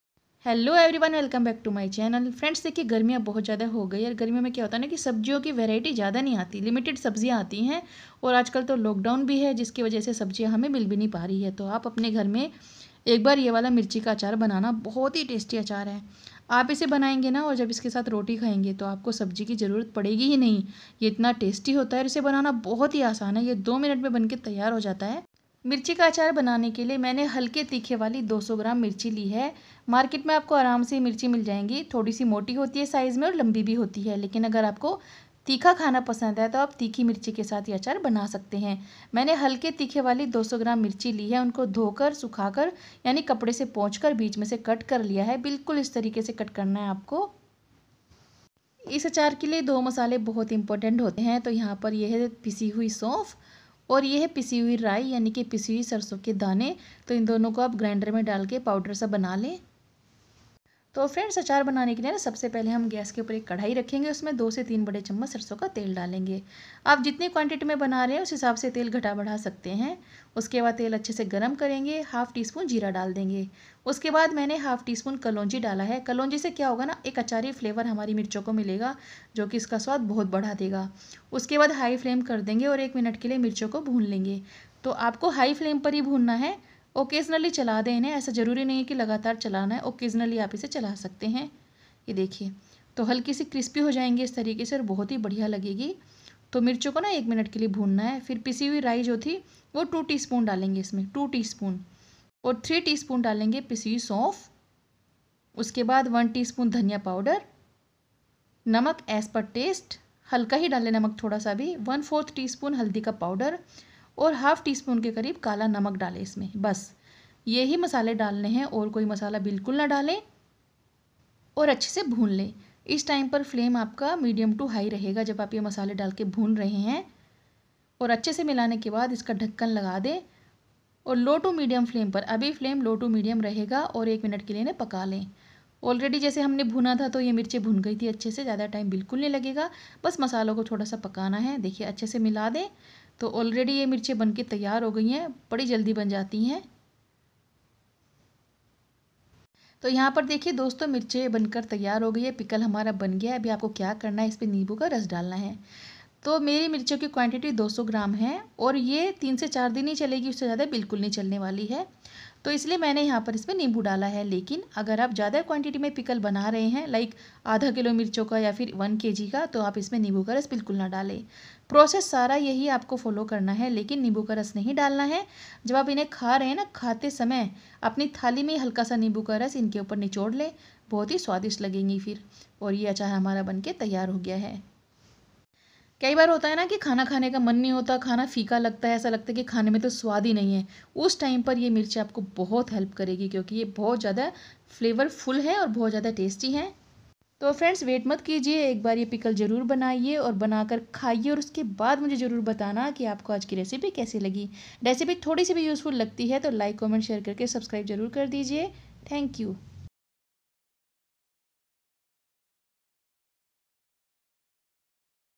2 मिनट मे बनाए हरी मिर्च का चटपटा अचार/बिना भूख सिर्फ इस हरी मिर्च से ही चार रोटी खालोगेchili pickle
2 मिनट मे बनाए हरी मिर्च की चटपटा अचार /बिना भूख सिर्फ इस हरी मिर्च से ही चार रोटी खालोगेchili pickle

hello family

Buying Links :

hello friends

hello friends

Buying Links :

Buying Links :

hello friends

jab khana lage beswad ,kuch bhi khane ka man na ho tab ye green chili pickle bhadayega apke khane ka swad
ek bar banana, khana Phir comment Karke jarur batana ki recipe Kaise lagi

INGREDIENTS  FOR 200 GMS CHILI'S

200 GMS GREEN CHILI
2 TSP MUSTARD POWDER
3 TSP FENNEL POWDER
1 TSP CORIANDER POWDER
1/4 TSP TURMERIC POWDER
1/2 TSP BLACK SALT
1/2 TSP CUMIN SEEDS
1/2 TSP ONION SEEDS
REGULAR SALT 1/4 TSP
LEMON JUICE

kitchen products link's